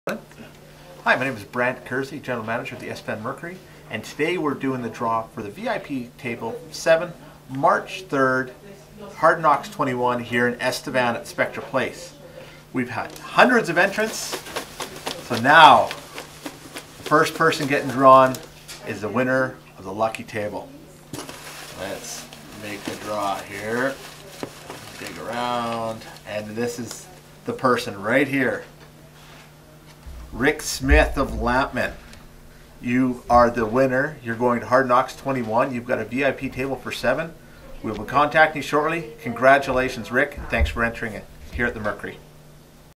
Estevan Mercury HK 21 draw
Over hundreds of people entered into our draw for the VIP table at Hardknocks 21, March 3 at Spectra Place. Watch the video to find out who won.